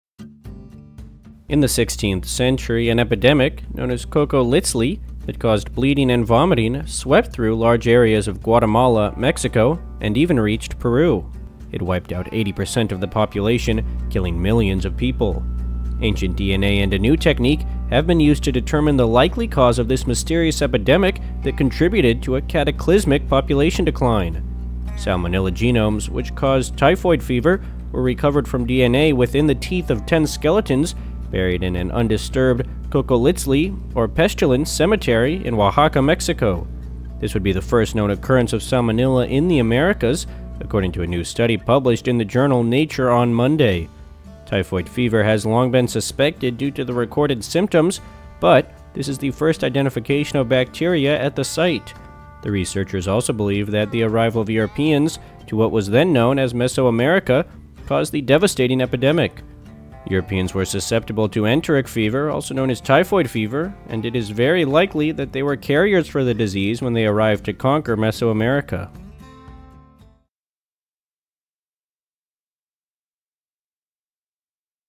16th century epidemic in Central America discovered
In the 16th century, an epidemic known as "cocoliztli" that caused bleeding and vomiting swept through large areas of Guatemala, Mexico and even reached Peru. It wiped out 80% of the population, killing millions of people. Ancient DNA and a new technique have been used to determine the likely cause of this mysterious epidemic that contributed to a "cataclysmic" population decline.